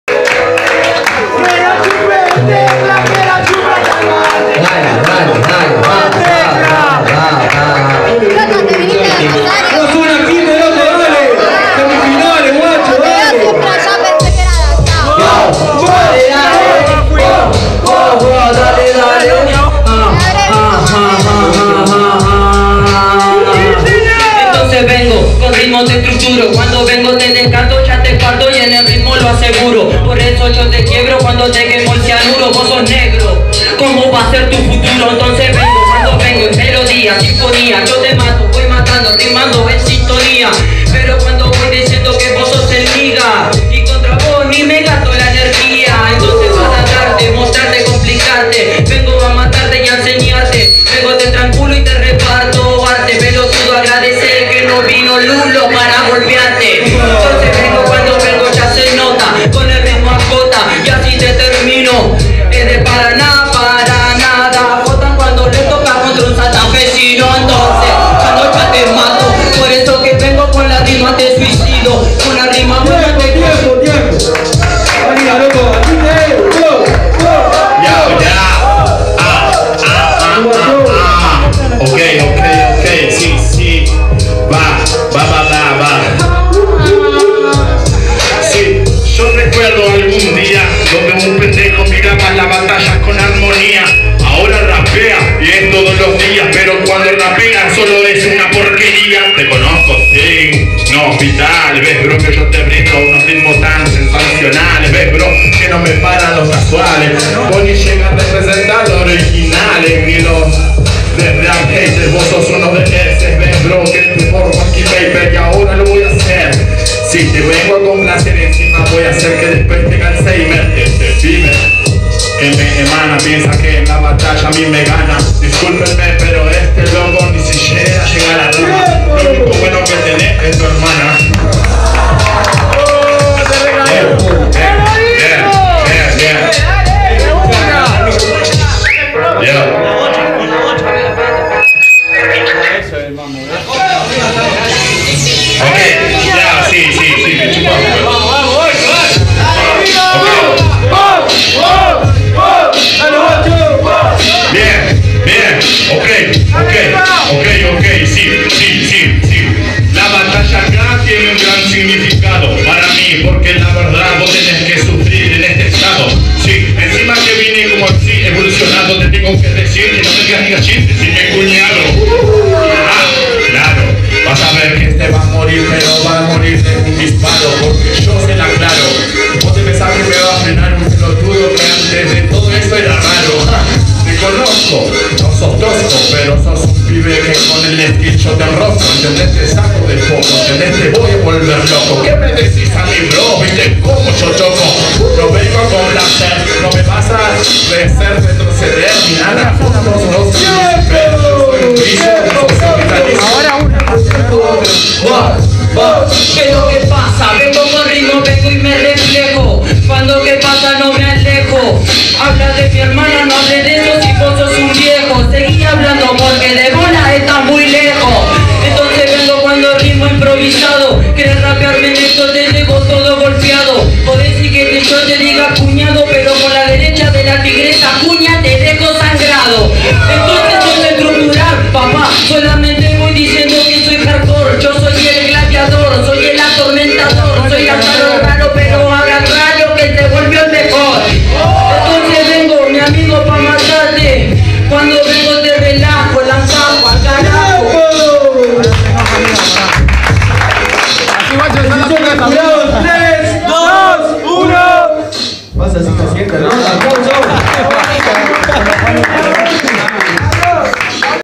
Signo vs Dual Nigga Semifinal Lenguas Sualtas 2da Edicion
Competencia: Lenguas Sueltas

Modalidad: 1 vs 1

Lugar:Santa Fe capital

Ganador: Signo